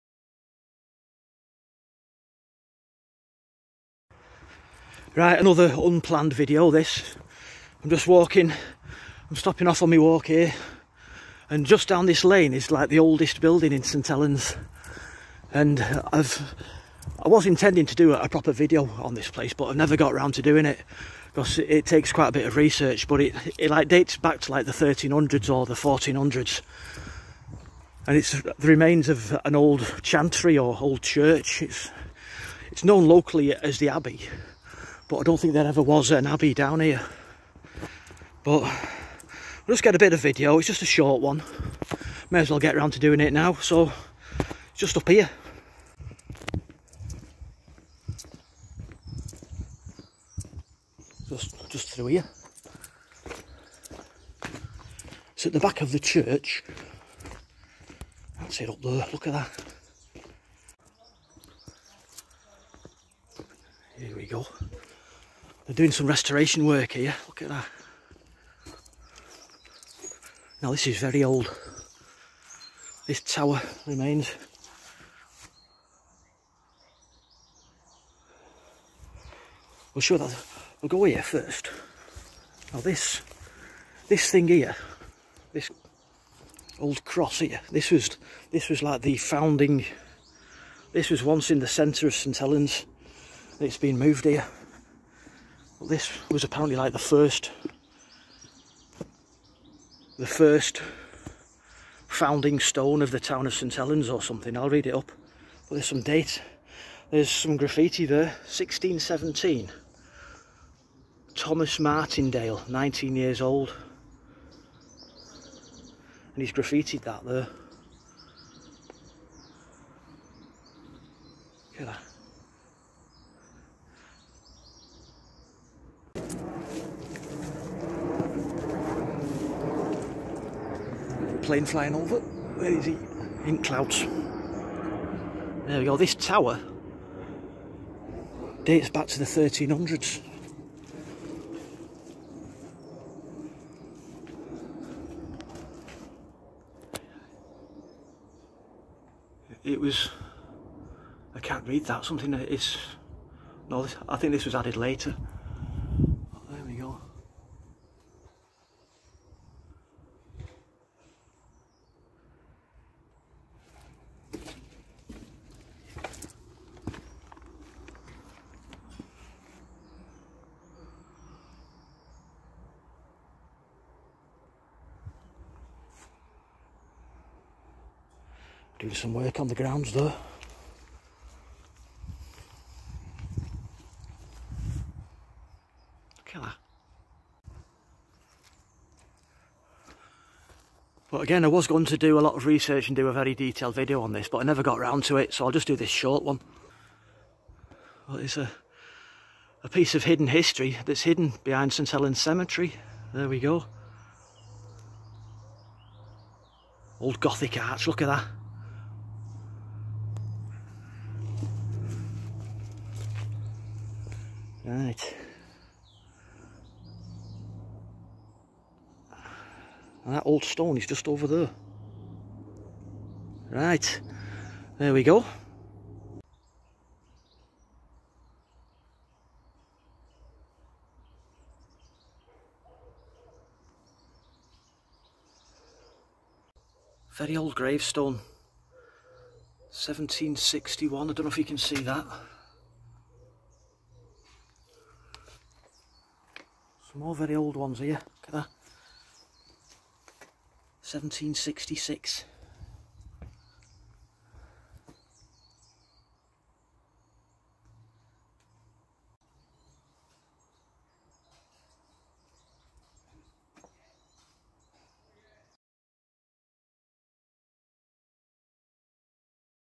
Windleshaw Chantry 1415, "The Abbey", oldest building in St.Helens.
A quick explore showing the oldest building in st.helens. Windleshaw "Abbey", 1415. Grade 2 listed building.

Thomas Martendale was the landlord of the Kings Head Inn in 1629, located in what is now Church Street, same location as the stone cross.